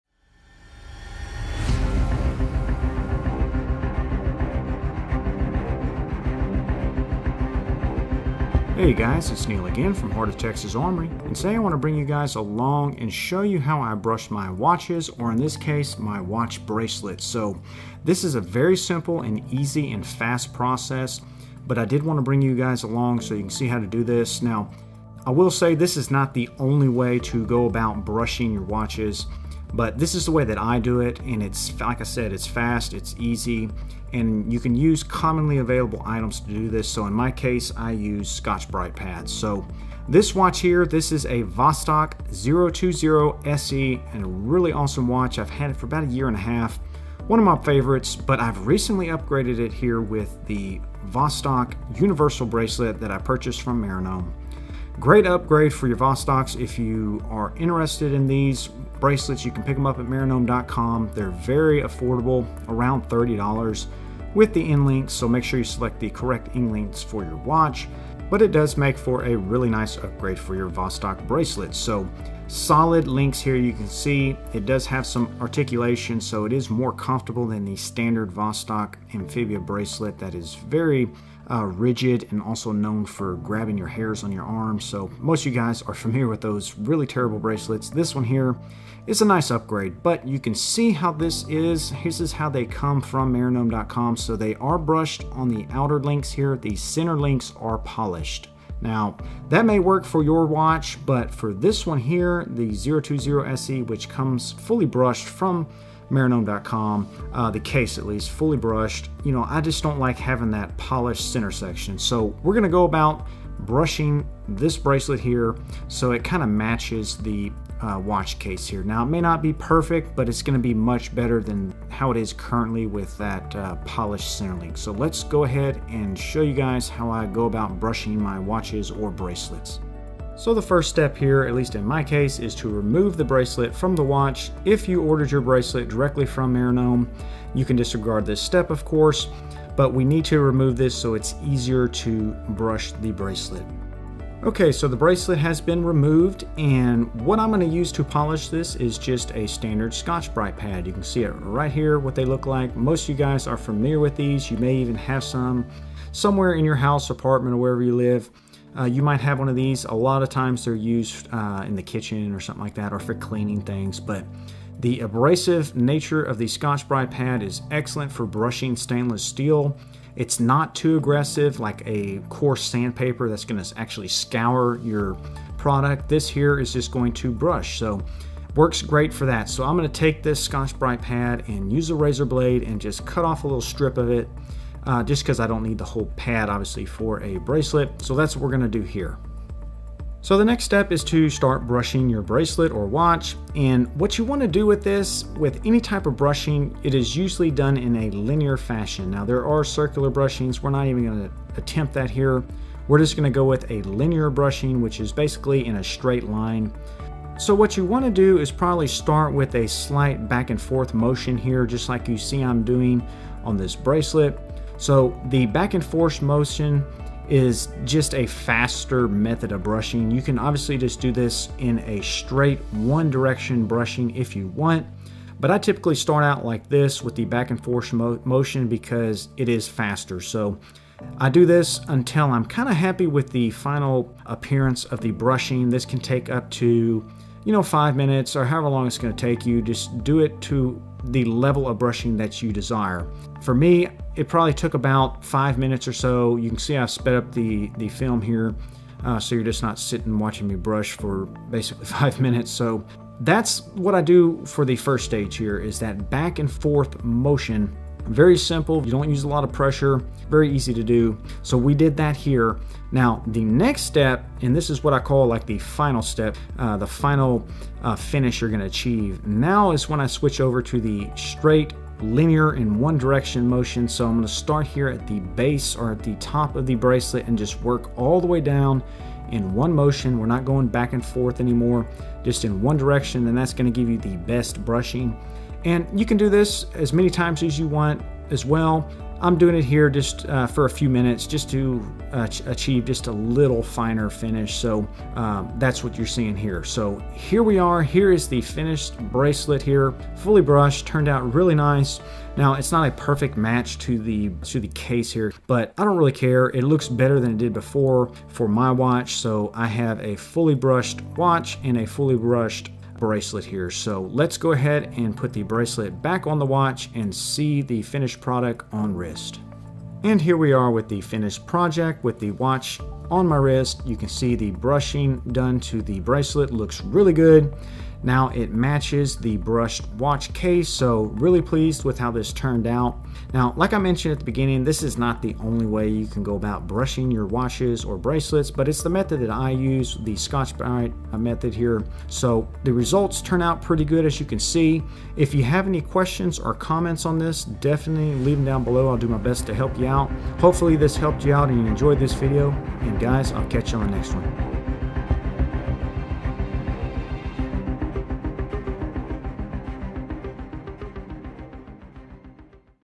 Let's Brush a Watch Bracelet - A Simple and Fast How To Guide
Today I show you how I easy archive a nice brushed finish on my new Universal Bracelet for my Vostok 020SE.  This method can be used for just about any watch  or watch accessory like a bracelet.  Works particularly great on stainless steel and bronze.  All you need is a few minutes of time and a Scotch-Brite pad.